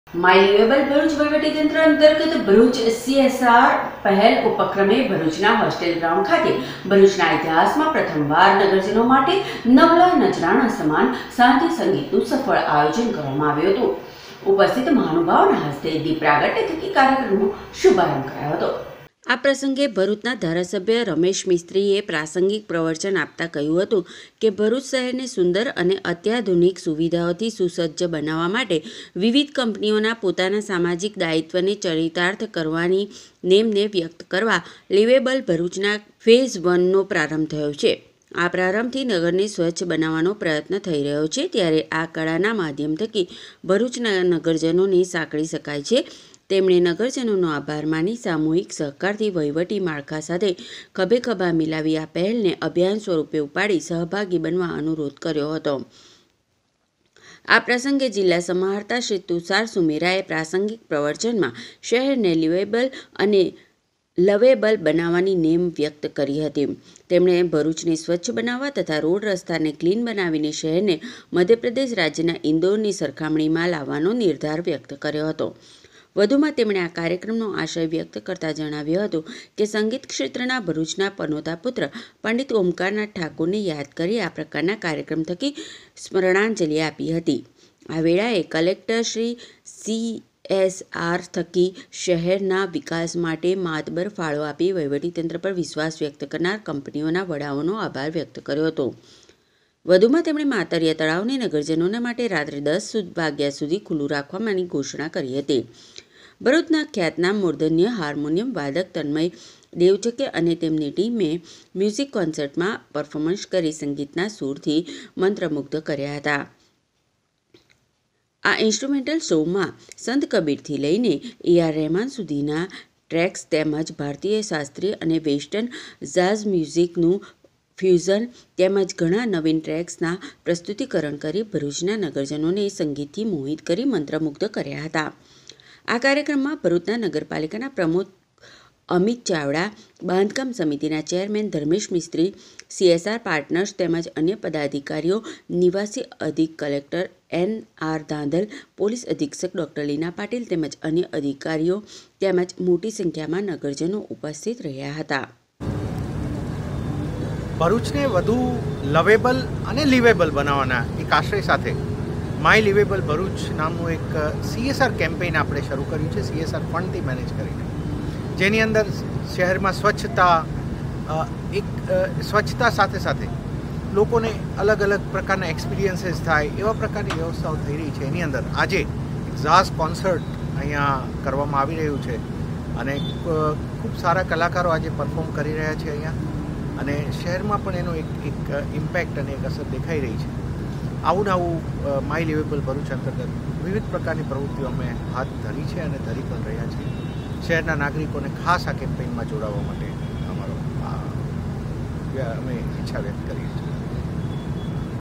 માઁ ટીવી.23 .01. 2023 ગીતપ્રેમી ભરૂચી નો જિલ્લા વહીવટીતંત્ર નવલા નજરાણાની અભુતપૂર્વ પ્રતિસાદ
સંગીત જલસો : ગીતપ્રેમી ભરૂચી નો જિલ્લા વહીવટીતંત્ર નવલા નજરાણાની અભુતપૂર્વ પ્રતિસાદ
માય લિવેબલ ભરૂચ અંતર્ગત જિલ્લા તંત્ર દ્વારા આયોજિત સાંધ્ય સંગીતમાં જિલ્લાવાસીઓ મોટી સંખ્યામાં ઉપસ્થિત રહ્યા : મૂર્ધન્ય  હારમોનિયમ વાદક  તન્મય દેવચકેનાં હાર્મોનિયમનાં સુરે નગરજનોને મંત્રમુગ્ધ કર્યા